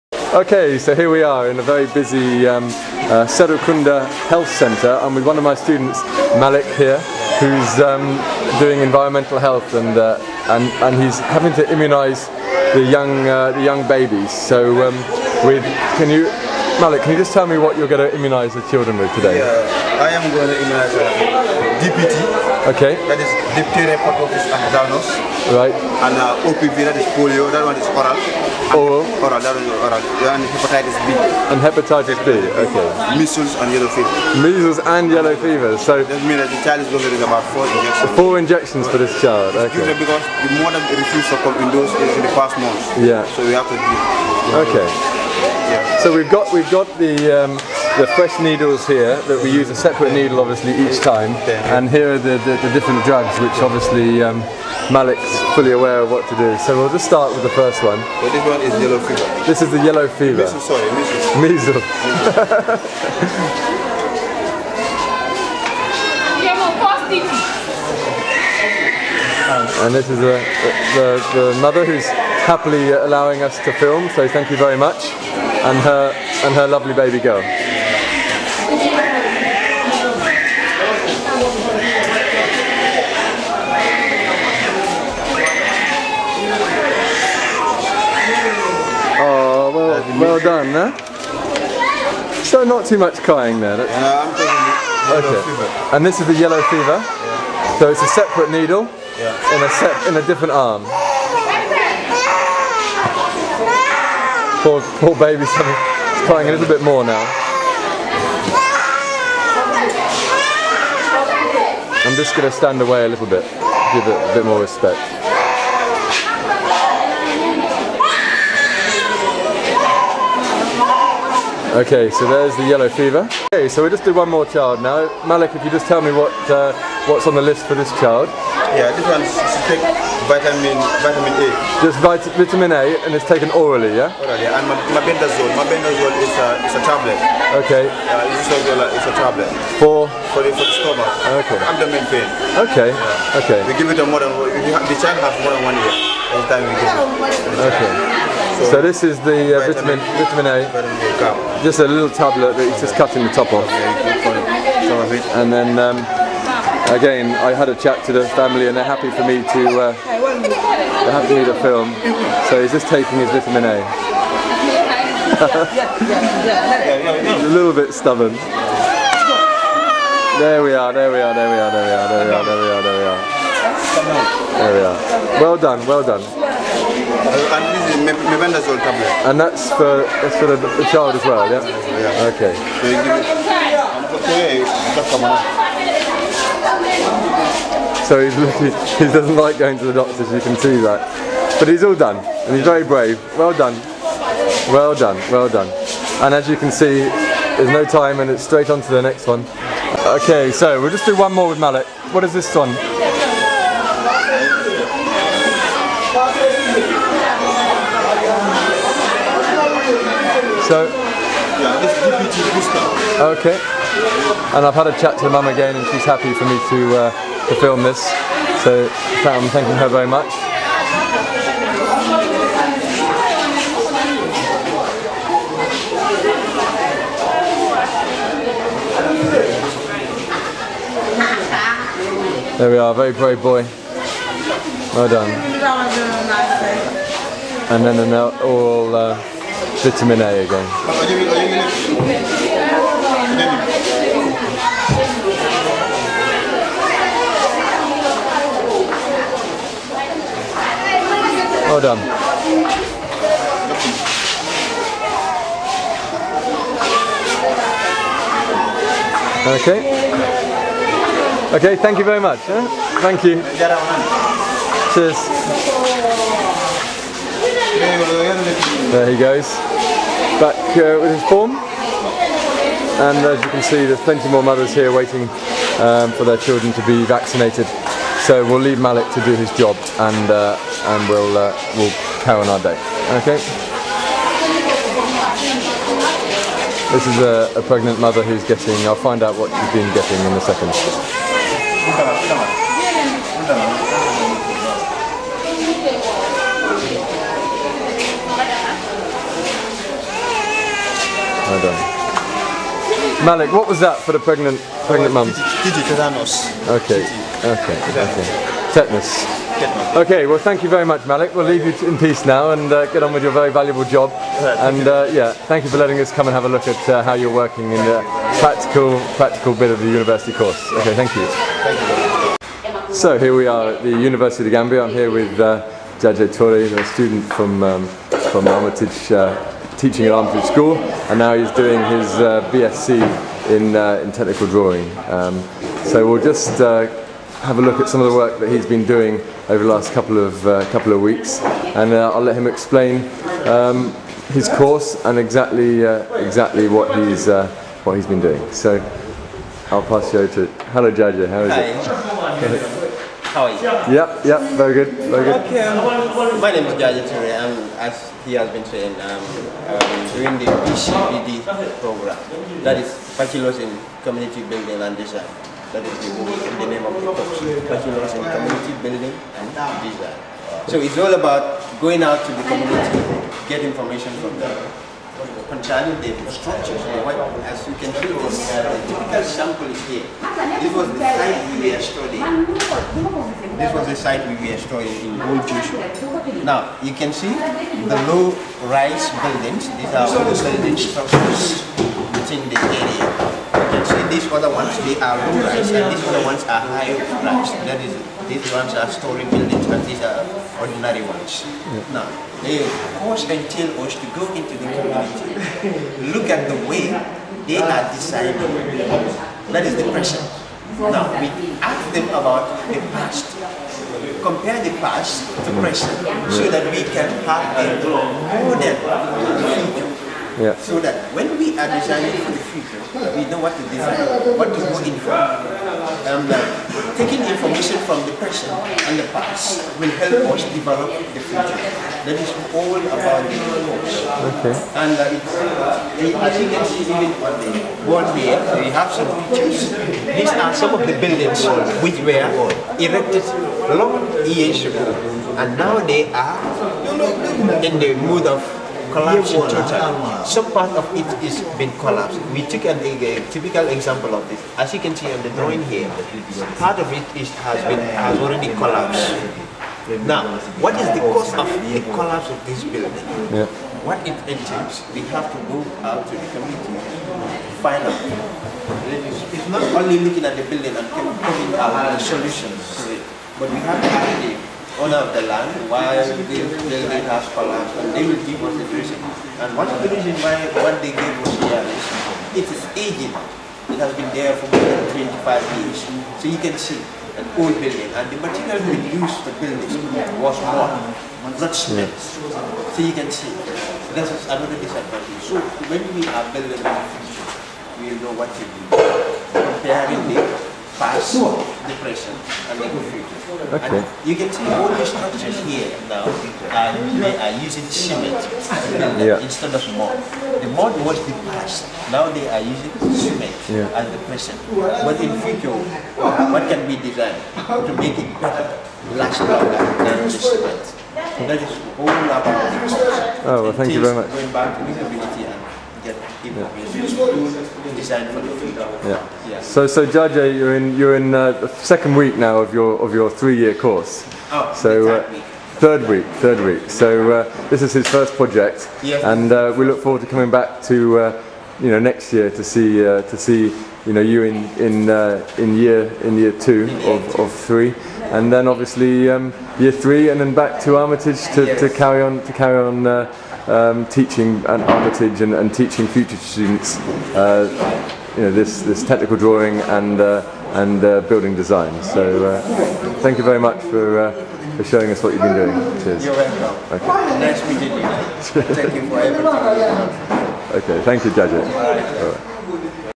University Students: The Georgetown Trust
The Georgetown Trust is a UK registered Charity sponsoring Gifted Gambians to go to University in the Gambia. This short video diary shows two of the students sponsored explaining their university course. More information on The Georgetown Trust can be found